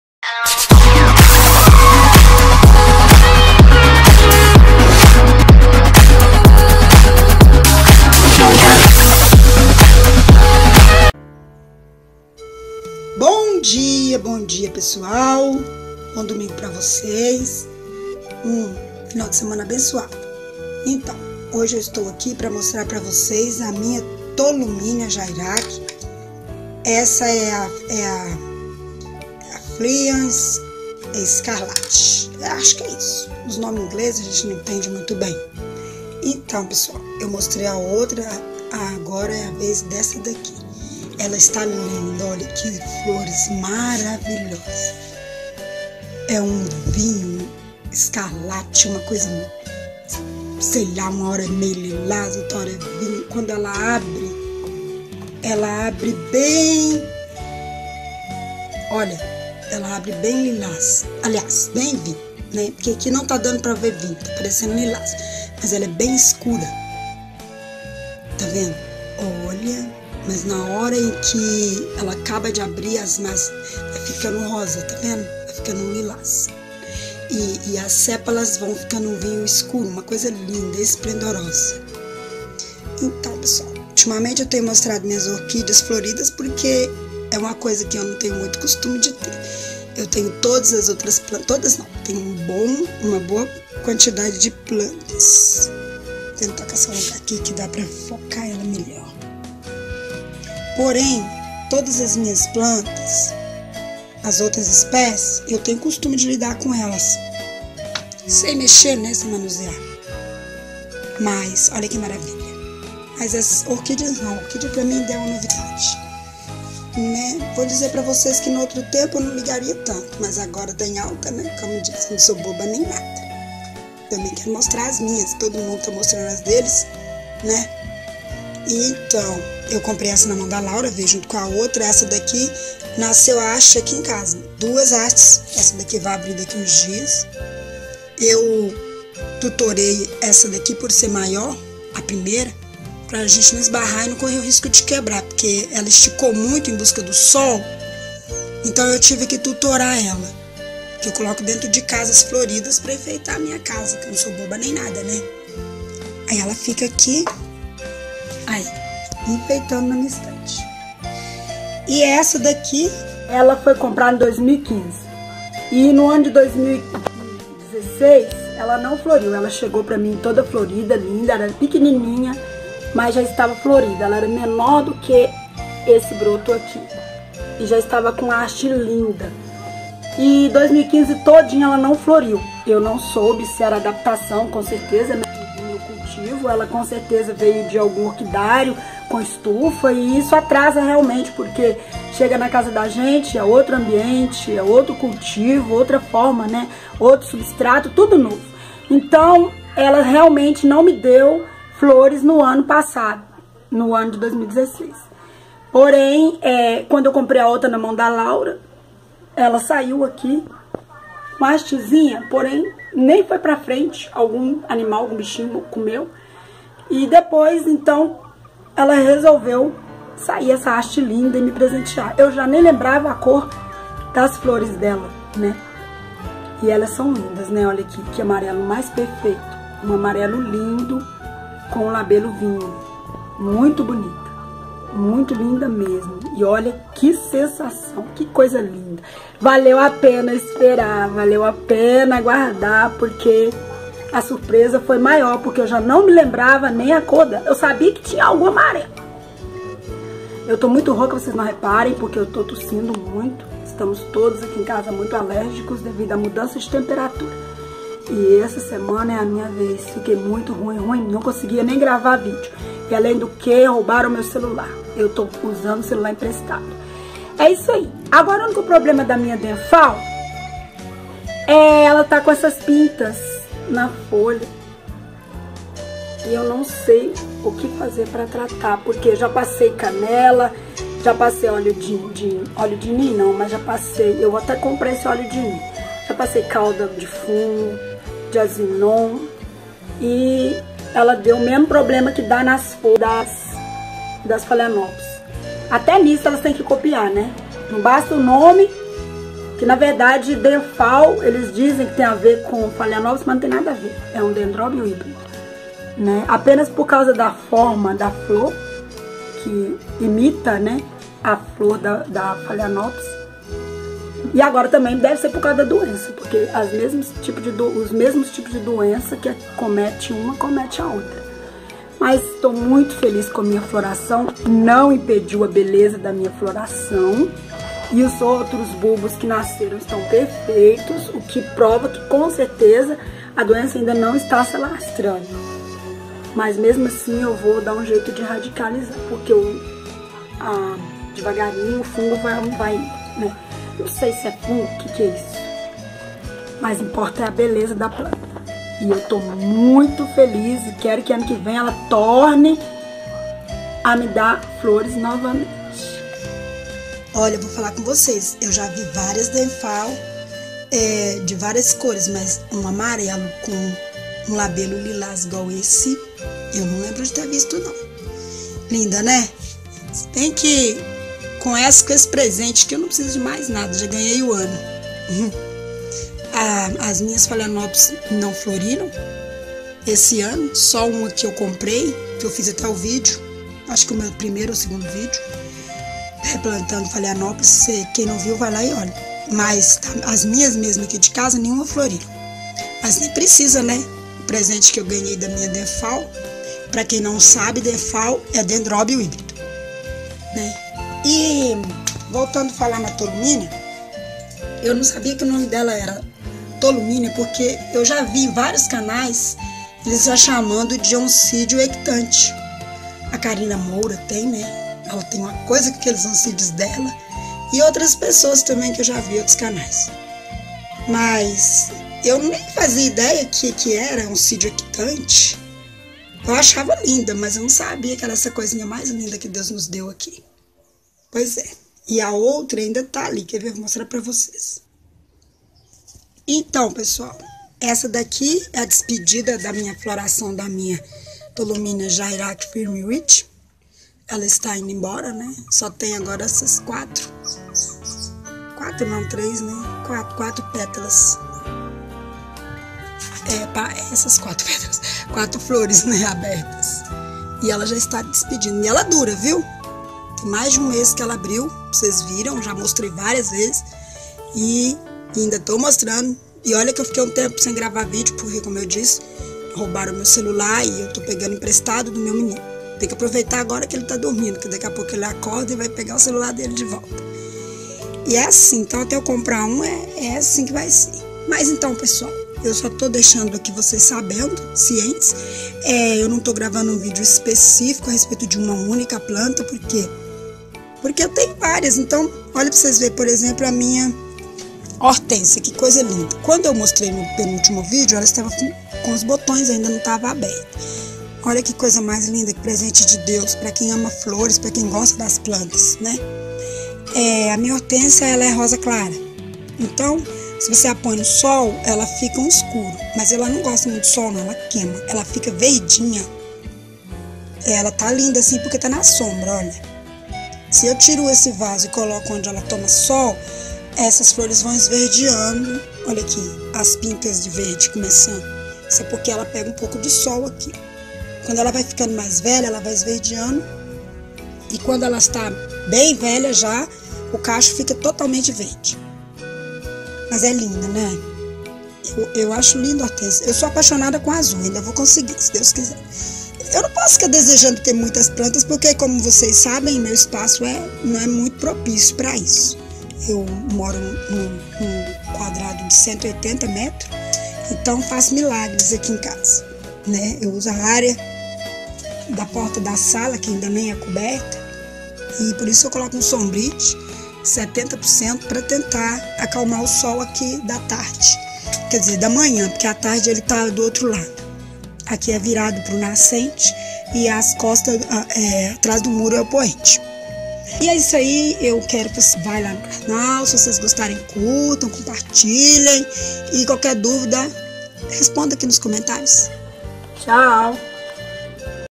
A floração das minhas orquídeas
Nesse vídeo mostro o desenvolvimento e floração das minhas orquídeas (Tolumnia Sairak flyer "scatter" e Denphal ( dendrobio hibrido amarela)